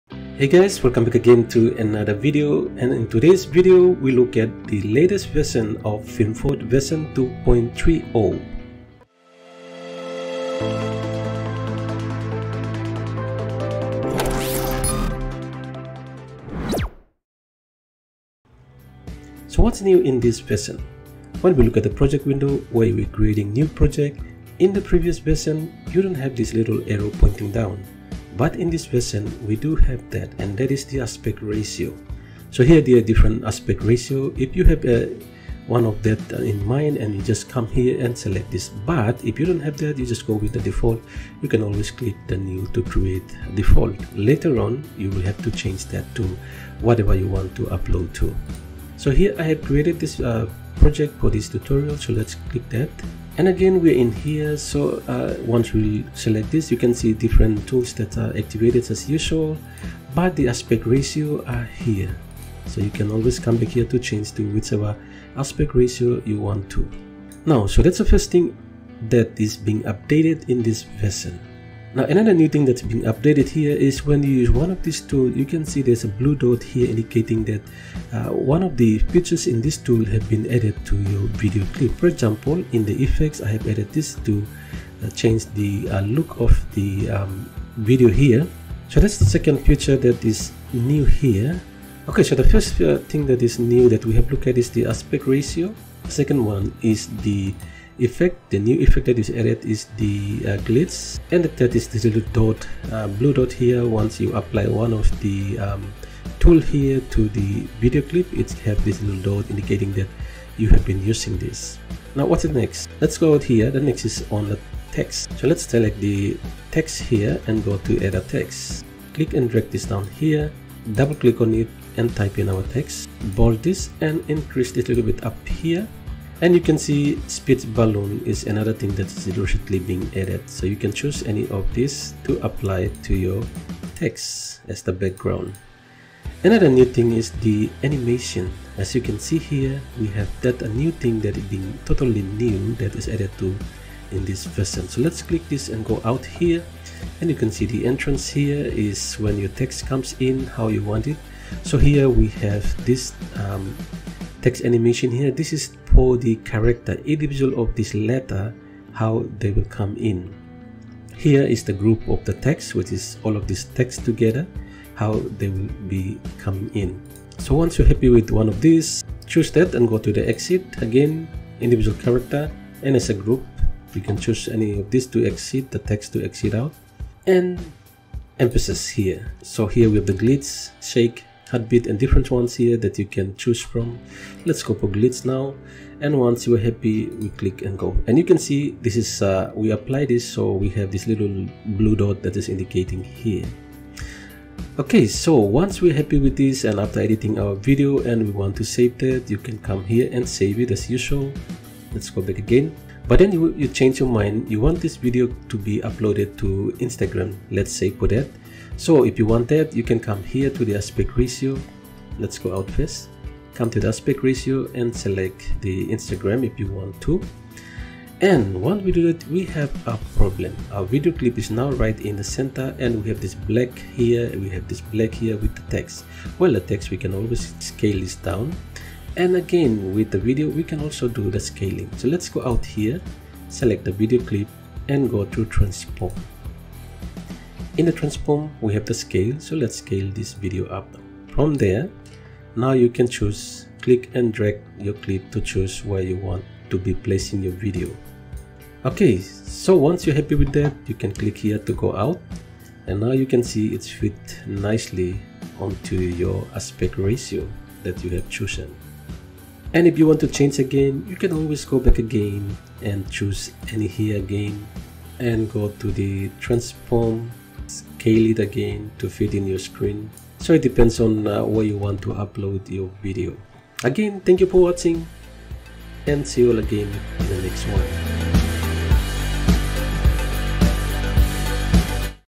New Text Animation in FilmForth Free Video Editor
FilmForth is an Easy and Powerful Free Video Editor. It  gives the options of Edit video from clips and add stylish transitions, filters, titles and more. What is more it is water mark free. With the latest update version 2.3.0, it brings yet beautiful text animation. So in this video we look at what's new in this updates version 2.3.0.